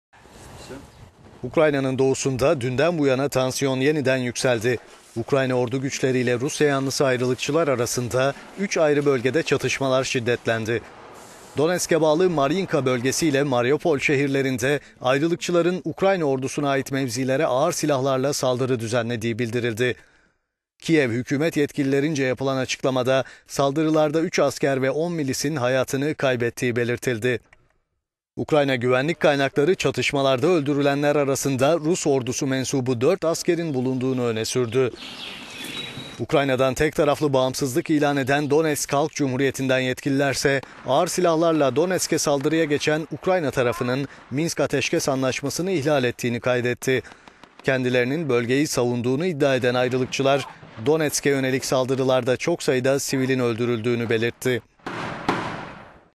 Ukrayna'nın doğusunda çatışmalar şiddetlendi
Ukrayna'nın doğusunda dünden bu yana tansiyon yeniden yükseldi. Ukrayna ordu güçleri ile Rusya yanlısı ayrılıkçılar arasında üç ayrı bölgede çatışmalar şiddetlendi.

Donetsk'e bağlı Maryinka bölgesi ile Mariupol şehirlerinde ayrılıkçıların Ukrayna ordusuna ait mevzilere ağır silahlarla saldırı düzenlediği bildirildi. Kiev hükümet yetkililerince yapılan açıklamada, saldırılarda üç asker ve 10 milisin hayatını kaybettiği belirtildi.